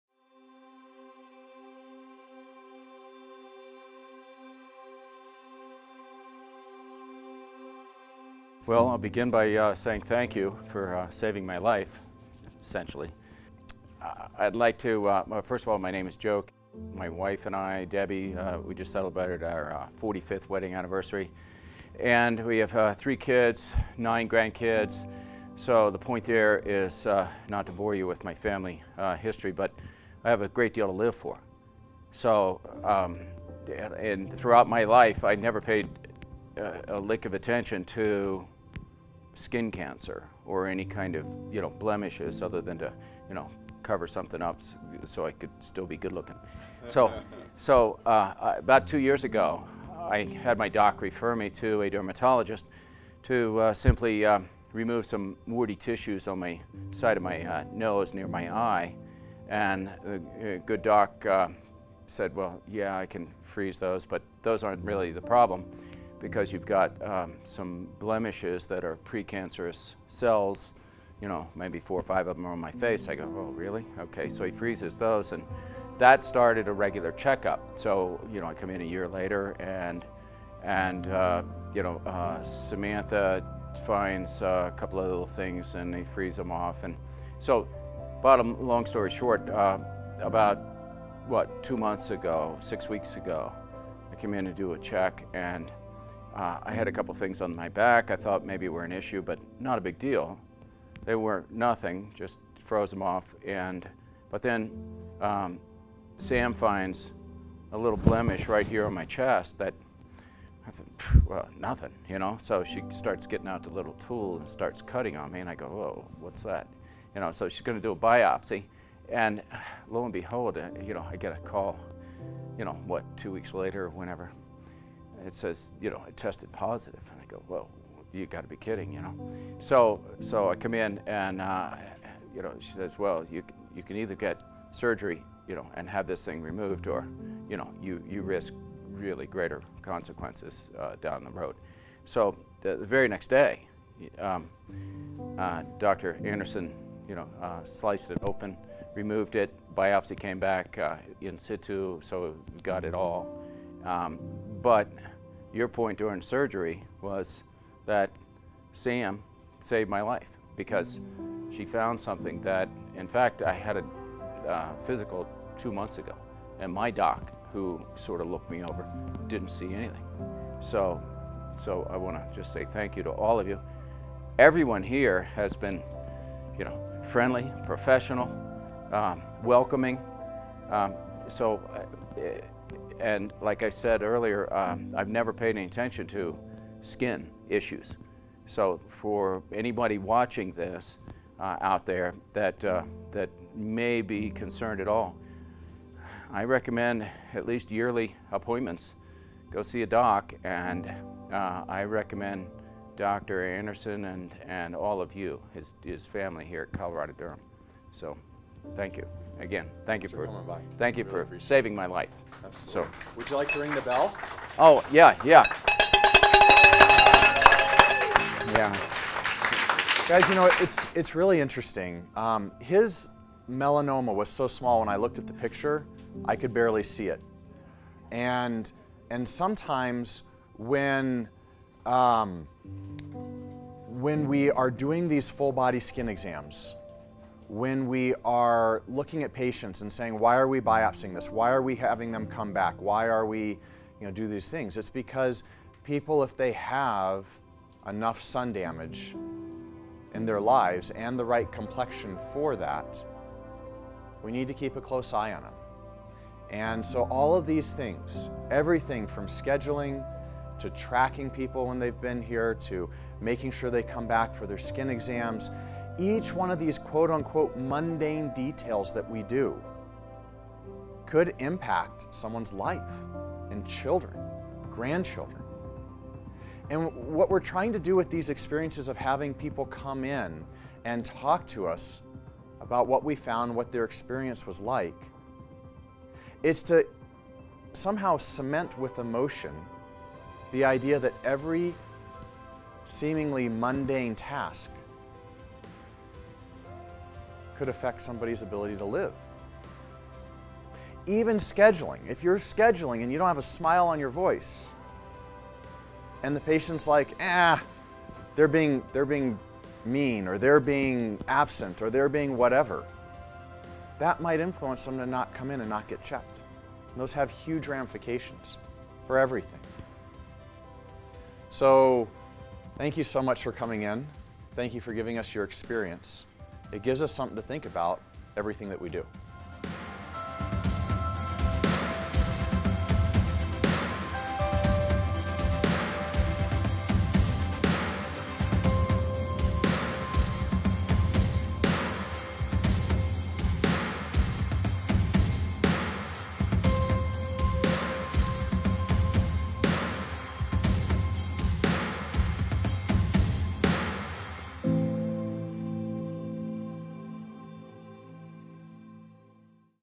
Melanoma Story [Testimonials] (2018)
In this video Dr. Reagan Anderson asks some patients to talk about their experience going through their melanoma skin cancer journey. He reminds his staff that the work they do matters and that patients are not defined by their condition or disease.

For any questions and access to our library of content

Video Production by Joshua Melendez

Partial Video Transcript:

"Patient: Well, I'll begin by saying thank you for saving my life, essentially. I'd like to, first of all my name is Joe. My wife and I, Debbie, we just celebrated our 45th wedding anniversary. And we have three kids, nine grandkids, so the point there is not to bore you with my family history, but I have a great deal to live for.

So, and throughout my life, I'd never paid a lick of attention to skin cancer, or any kind of you know, blemishes, other than to, you know cover something up so I could still be good-looking. So, so, about two years ago, I had my doc refer me to a dermatologist, to simply remove some --- tissues on my side of my nose, near my eye, and the good doc said well, "Yeah, I can freeze those but those aren't really the problem. Because you've got some blemishes that are pre-cancerous cells." You know, maybe four or five of them or on my face. I go, "Really? Ok." So he freezes those, and that started a regular checkup..."